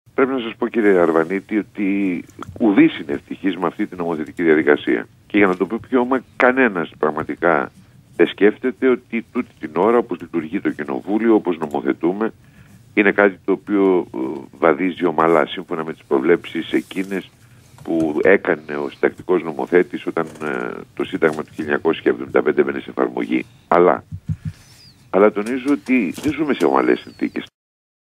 Ο Προκόπης Παυλόπουλος στο Κόκκινο 105,5
Ο Προκόπης Παυλόπουλος παραδέχεται, συνομιλώντας με τον Κώστα Αρβανίτη, ότι οι διαδικασίες που είχε προβλέψει ο συνταγματικός νομοθέτης δεν ακολουθούνται πλέον στην ελληνική Βουλή.